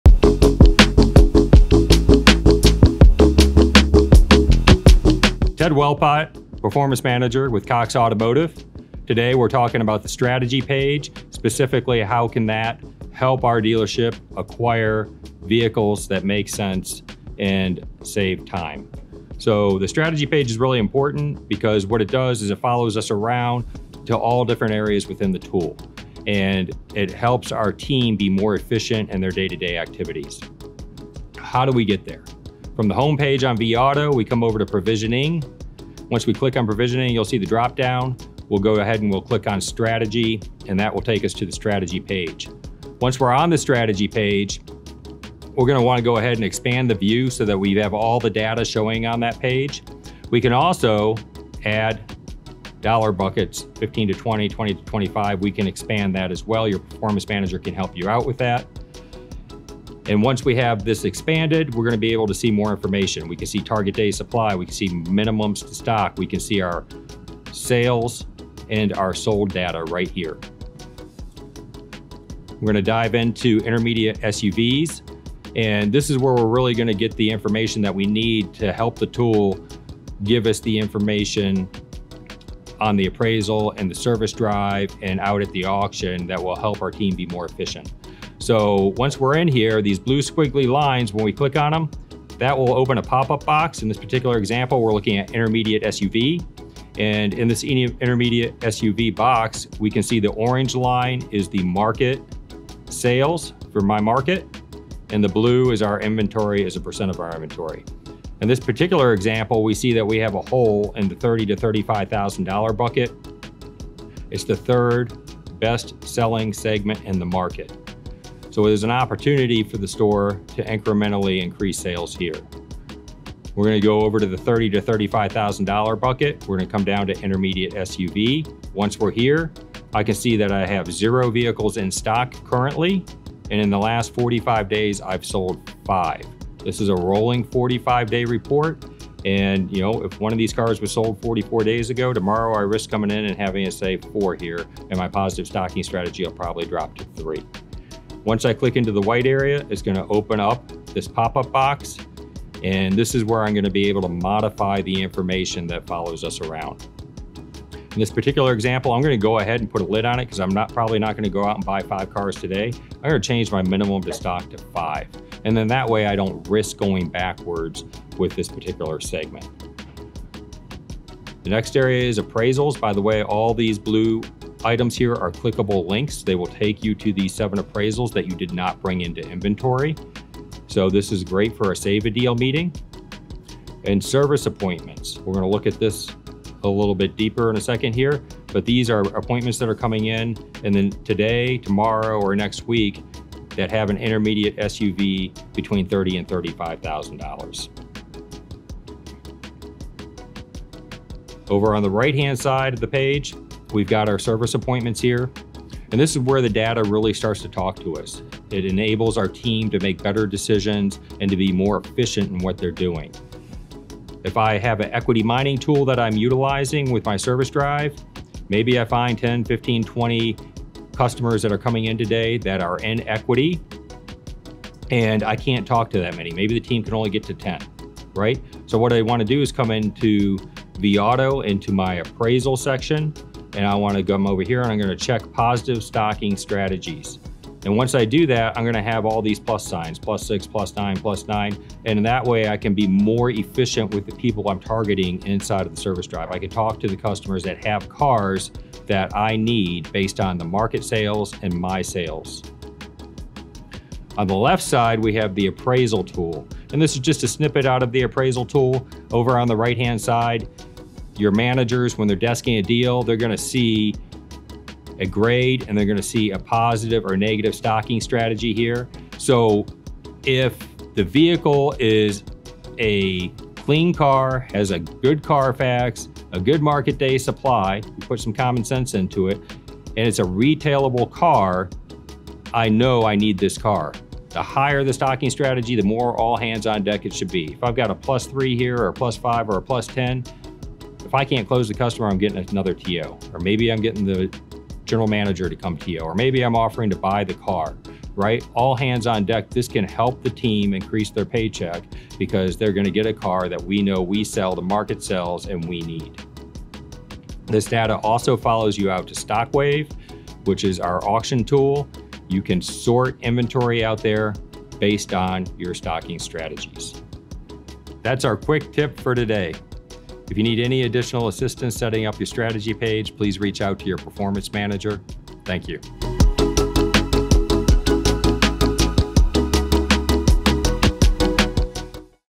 Quick Tip: vAuto Stocking Strategy
Explore how to take full advantage of vAuto’s Stocking Strategy with this step-by-step guide, which explains how to access and utilize the strategy page to make more efficient decisions when it comes to vehicle acquisition. See how these features can help you make informed inventory decisions that can ultimately increase your sales.